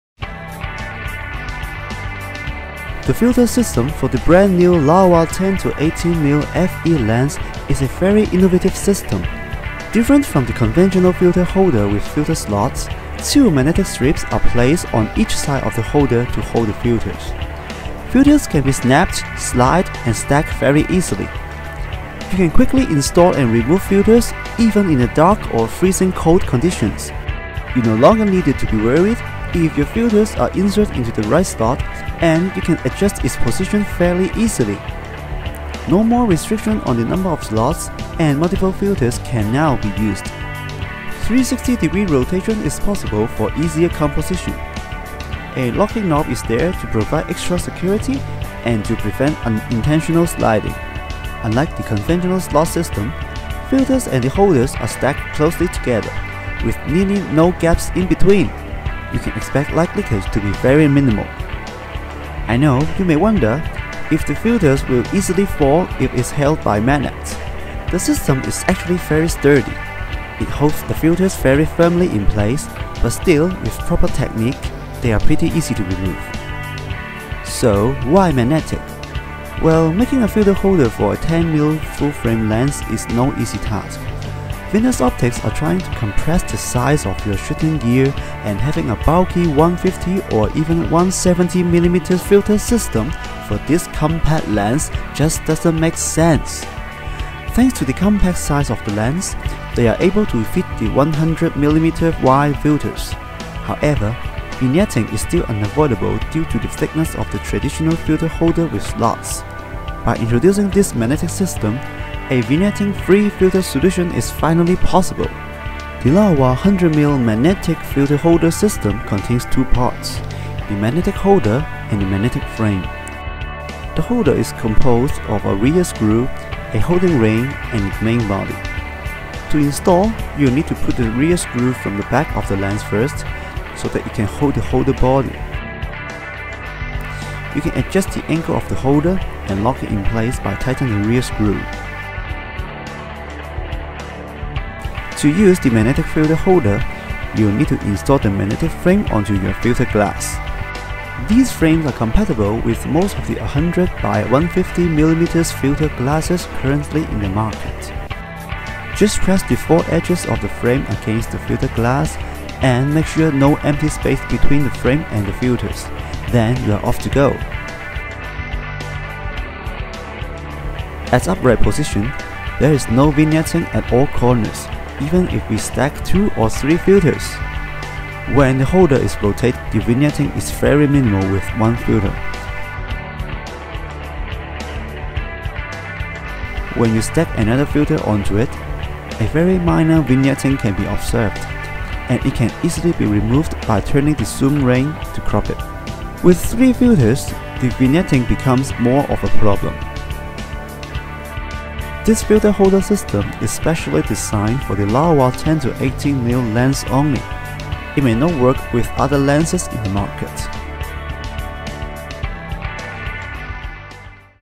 Introducing the new Laowa 100mm magnetic front filter holder for 10-18mm f/4.5-5.6 FE Zoom
A new Laowa 100mm magnetic front filter holder has been developed to fit 100mm-wide slot-in filters. Unlike conventional filter holder system with slots to slide the 100mm filters in, we have developed a magnetic filter holder that you can easily clip the 100mm GND filters onto the holder.

This not only removes all the troubles when installing a filter, it also reduces the vignetting arisen from using multiple filters. The magnetic holder can fit 2pcs of 100mm-wide filters without light getting blocked at any angles.